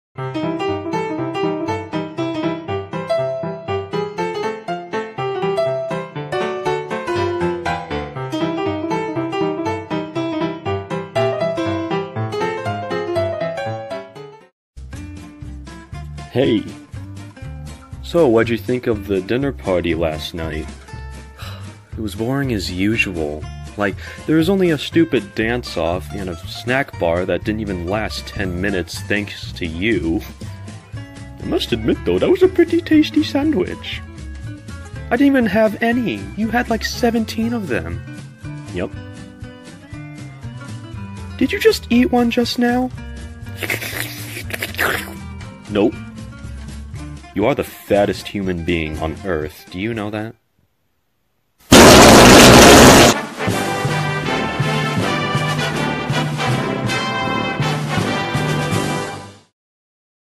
The Dinner Party [ANIMATION]
Pilot for my upcoming series, The Holliswood Forest.

Starring:
Harries Truman (skinny guy)
France Axelmeyer (fat guy)